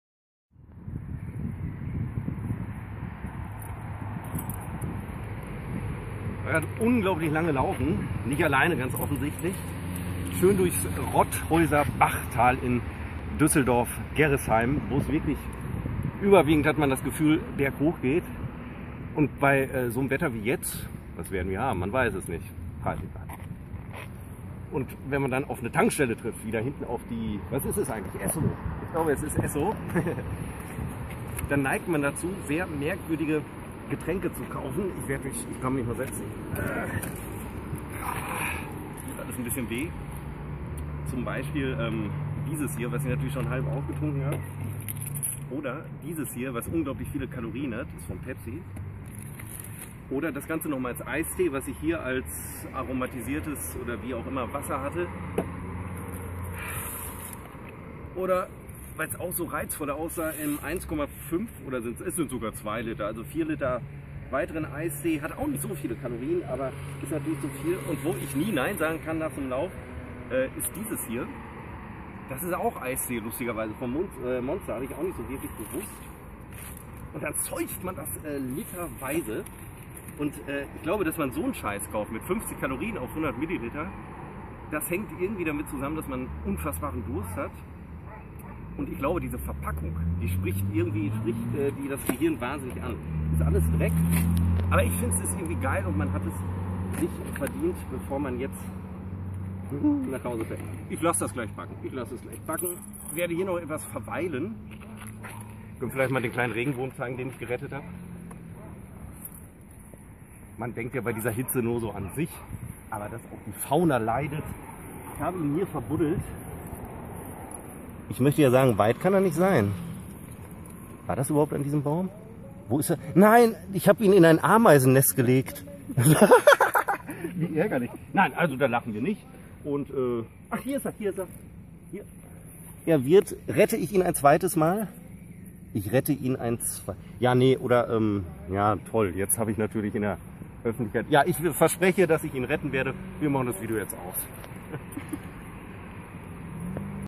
Getränke-Orgie nach Hitze-Lauf
04.06.15, nach einem Hitze-Lauf durch das Rotthäuser Bachtal
(hochgeladen für seppolog.de)